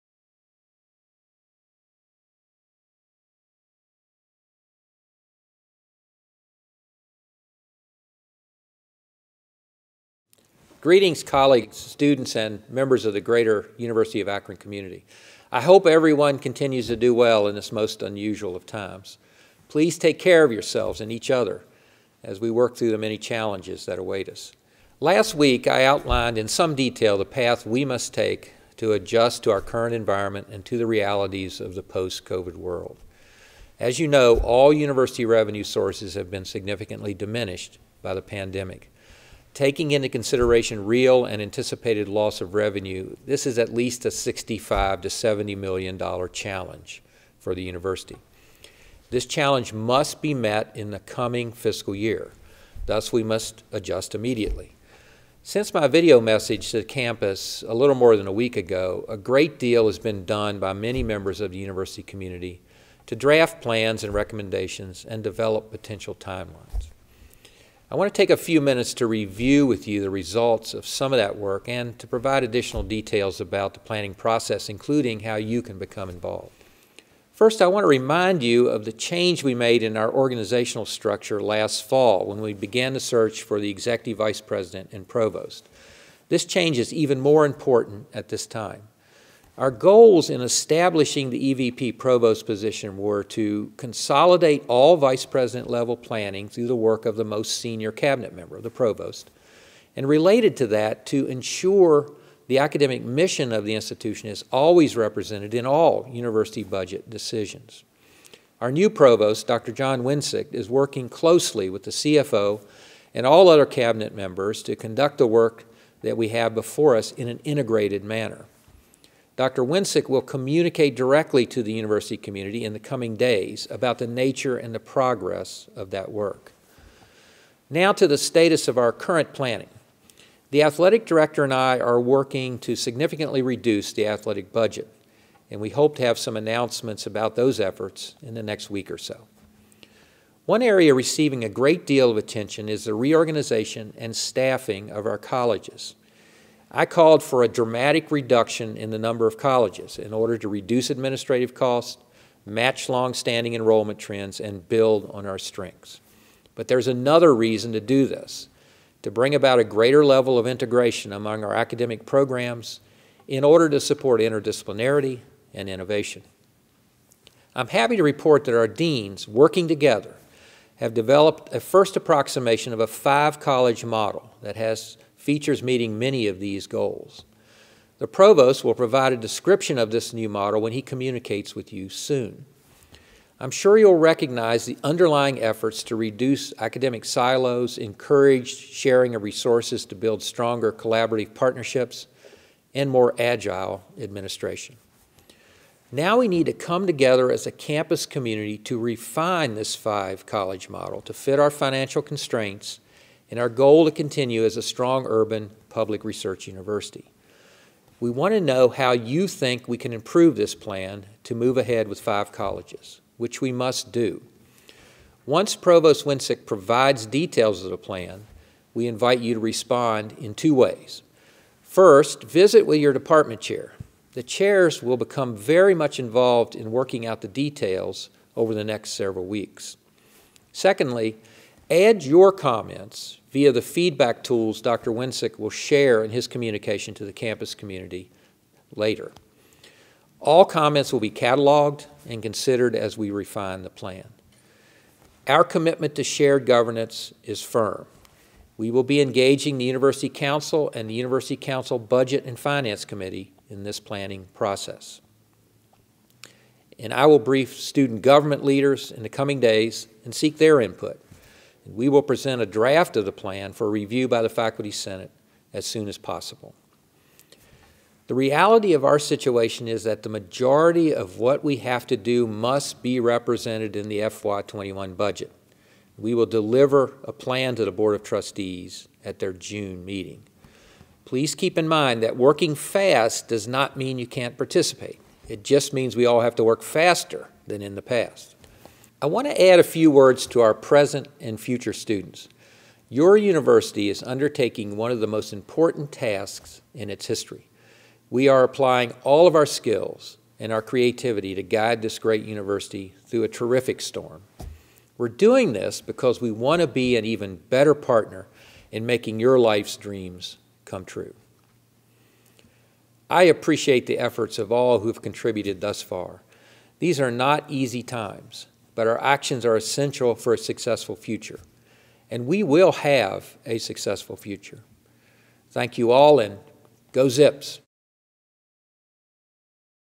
Redesigning The University of Akron: May 4, 2020 Update
We want to share an update with you regarding the redesign work completed to date and details about the planning process, including how you can be involved.